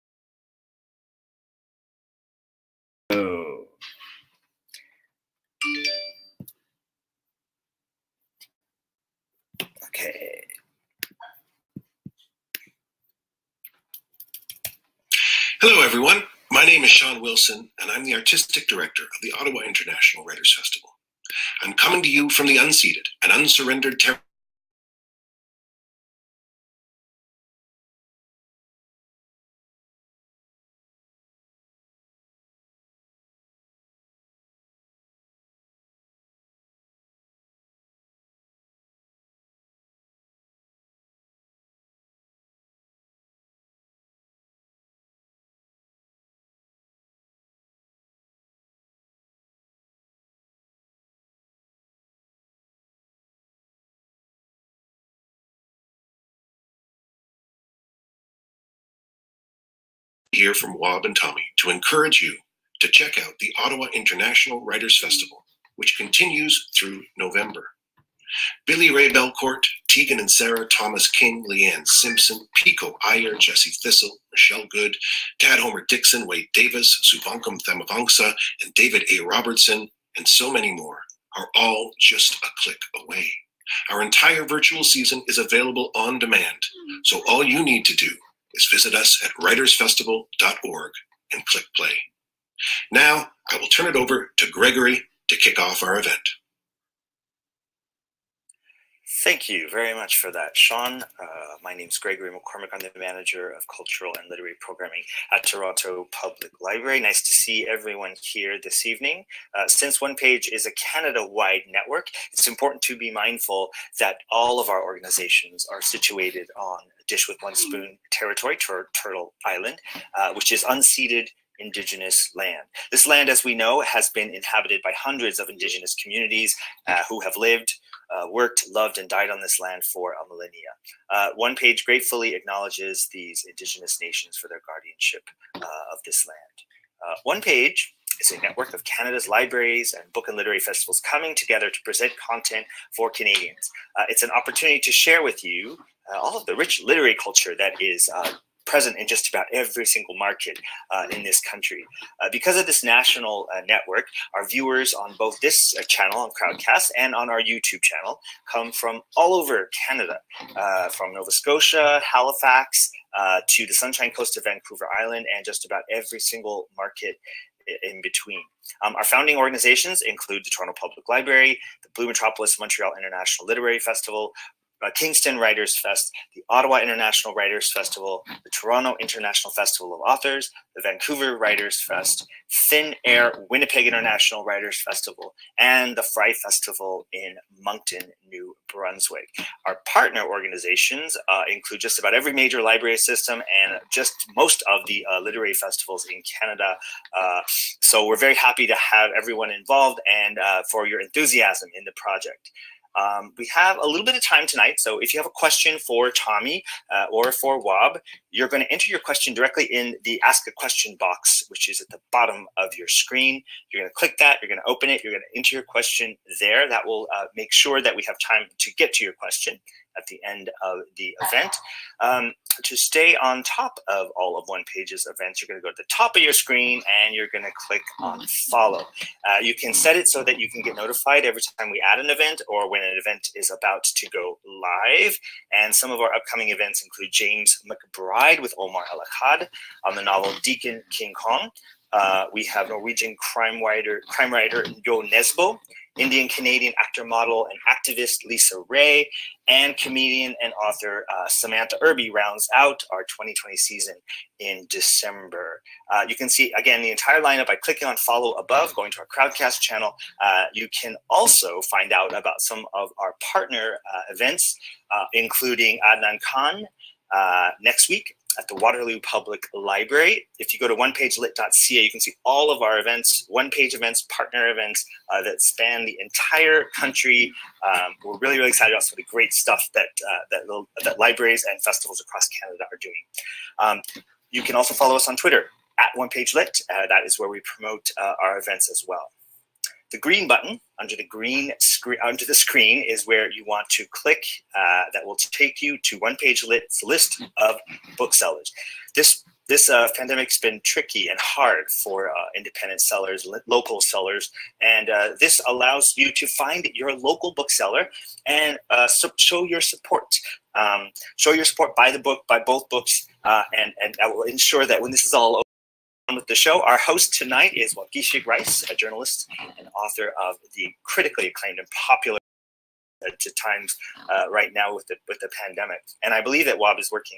Tommy Orange and Waubgeshig Rice: There There
This event is going like on October 29, 2020 at 9pm EDT.
In a sweeping yet nuanced exploration of identity and the role of Native Americans in a contemporary urban America (in this case, Oakland, California), Cheyenne and Arapaho author, Tommy Orange’s novel There There  presents a complex portrait of urban life and the impact that racism, ignorance and historical wrongs has had on an interconnected group of Native American characters. Inspired by a Gertrude Stein line about Oakland and all the ways it changed upon her return (“there is no there there”), Orange plays with this paradox of cities being both permanent and ever-changing places of nostalgia, desperation and success. The best-selling novel, one of America’s most critically acclaimed works of fiction in recent years, was a finalist for the 2019 Pulitzer.

Tommy Orange talks with Anishnaabe journalist and author Waubgeshig Rice (Moon of the Crusted Snow).